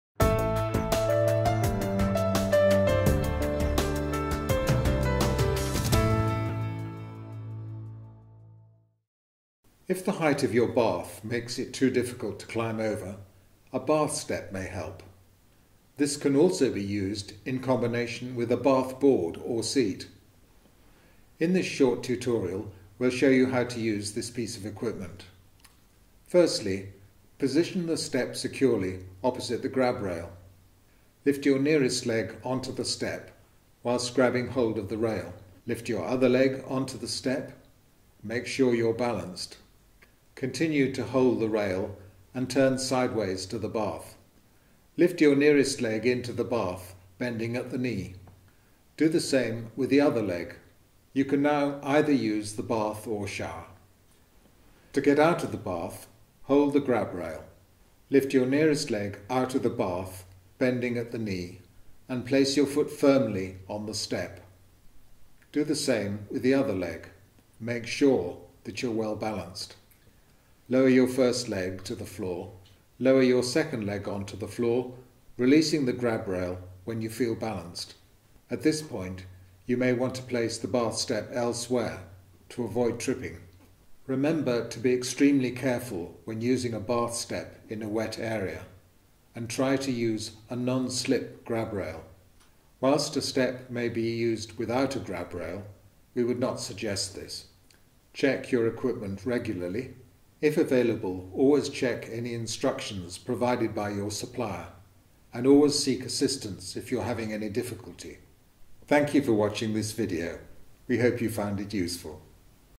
How To Use a Bath Step (HD)
A short video tutorial on how to use a bath step safely.